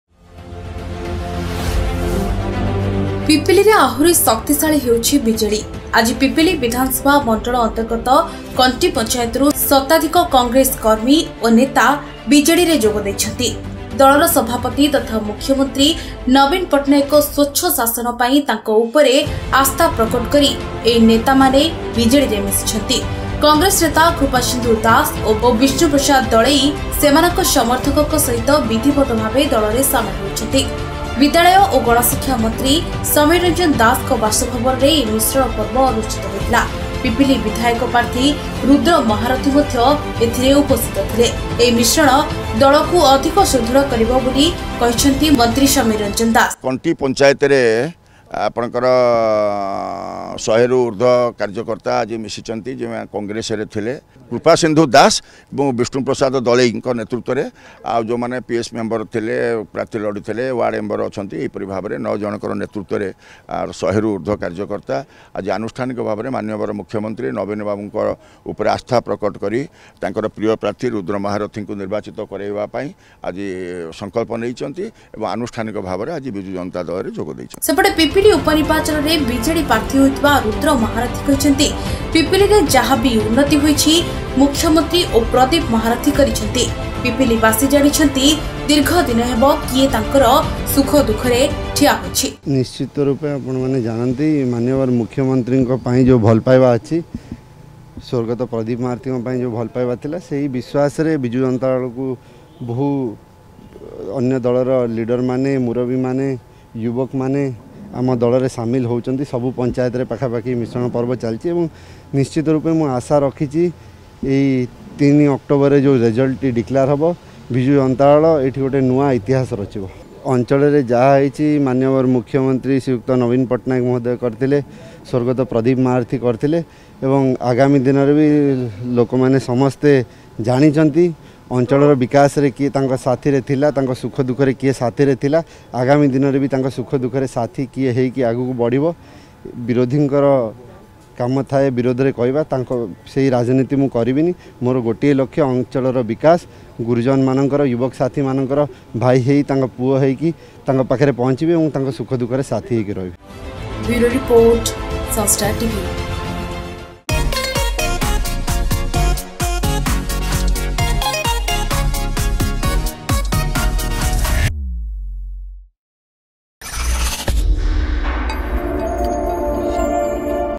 Pipili by Poll Update ; ପିପିଲିରେ ବଢ଼ୁଛି ବିଜେଡିର ବଳ, ଦଳରେ ସାମିଲ୍ ହେଲେ ଶତାଧିକ କଂଗ୍ରେସ କର୍ମୀ ଓ ନେତା
ପିପିଲିରେ ଆହୁରି ଶକ୍ତିଶାଳୀ ହେଉଛି ବିଜେଡି । ଆଜି ପିପିଲି ବିଧାନସଭା ମଣ୍ଡଳି ଅନ୍ତର୍ଗତ କଣ୍ଟି ପଞ୍ଚାୟତରୁ ଶତାଧିକ କଂଗ୍ରେସ କର୍ମୀ ନେତା ବିଜେଡିରେ ଯୋଗ ଦେଇଛନ୍ତି । ଦଳର ସଭାପତି ତଥା ମୁଖମନ୍ତ୍ରୀ ନବୀନ ପଟ୍ଟନାୟକଙ୍କ ସ୍ୱଚ୍ଛ ଶାସନ ପାଇଁ ତାଙ୍କ ଉପରେ ଆସ୍ଥା ପ୍ରକଟ କରି ଏହି ନେତାମାନେ ବିଜେଡିରେ ମିଶିଛନ୍ତି